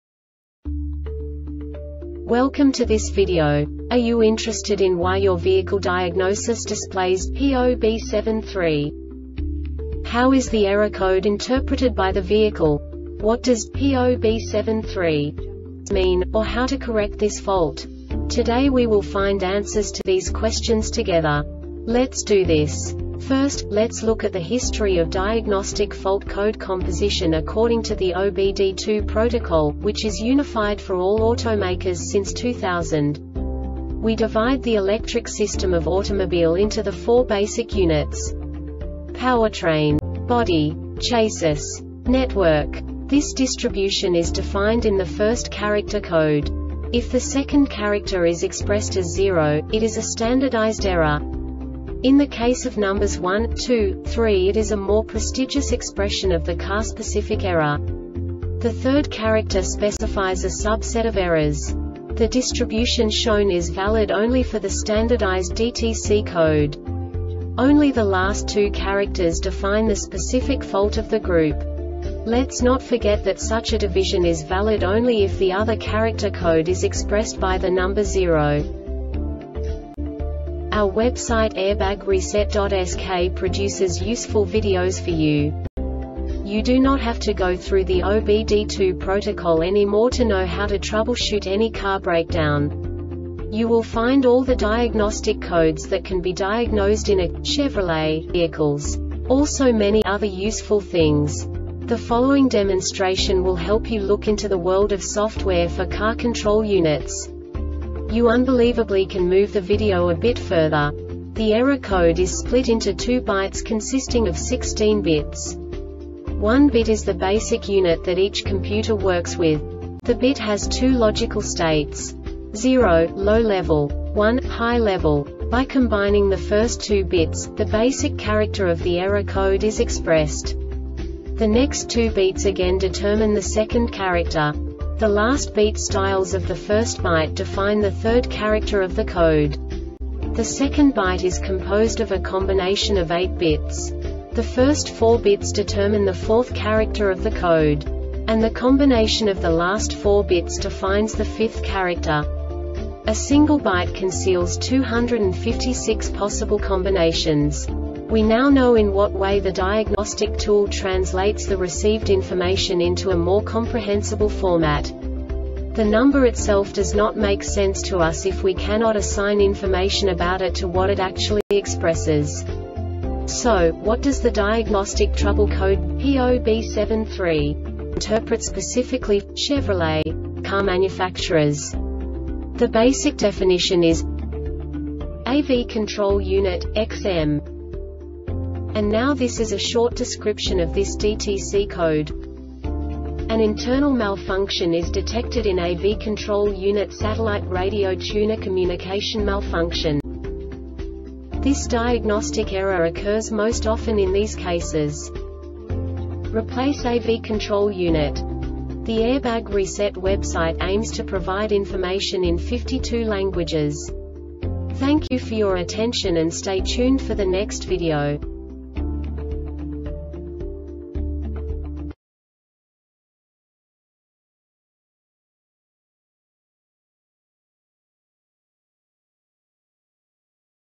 DTC Chevrolet P0B73 Short Explanation
Contents:
0:21 Basic DTC analysis according to OBD2 protocol standard.
1:48 Insight into programming
2:58 A diagnostically understandable description of the Diagnostic Trouble Code
3:05 Basic error definition

Make:
Chevrolet
Code:
P0B73
Definition:
AV Control Unit - XM
Description:
An internal malfunction is detected in AV control unit (satellite radio tuner communication malfunction).
Cause:
Replace AV control unit.